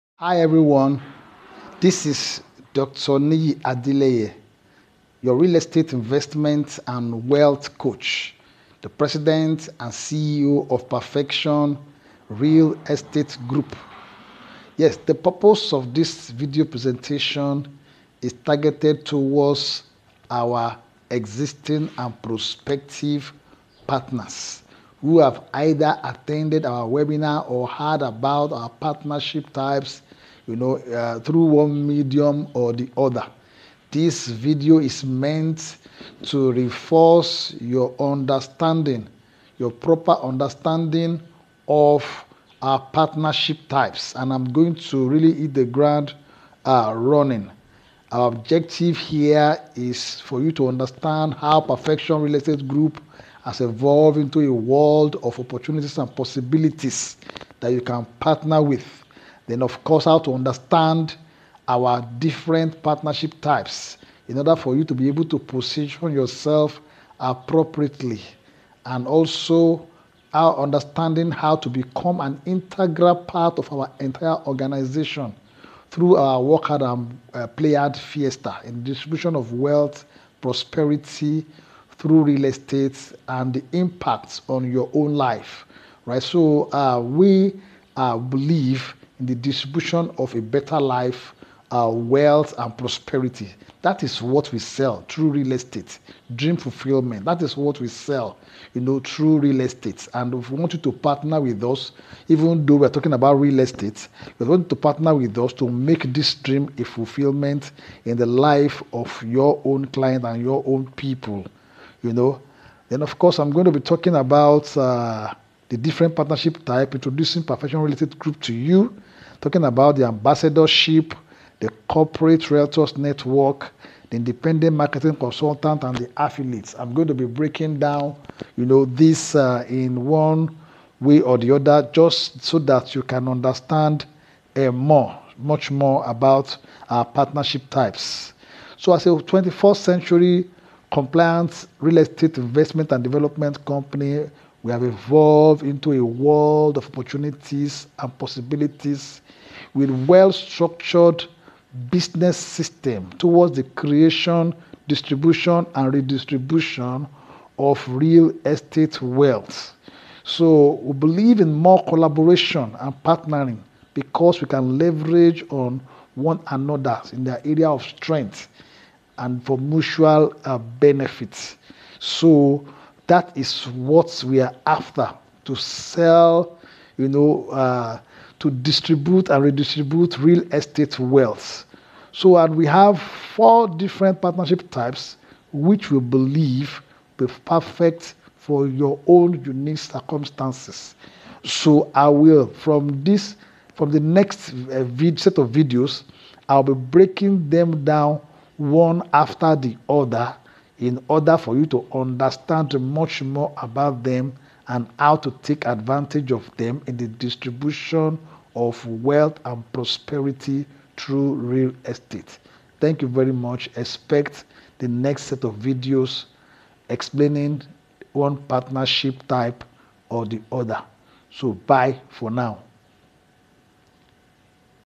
PREG PARTNERSHIP TYPES
Understanding the Perfection Real Estate Group Partnership types for the purpose of positioning yourself appropriately into what works for you.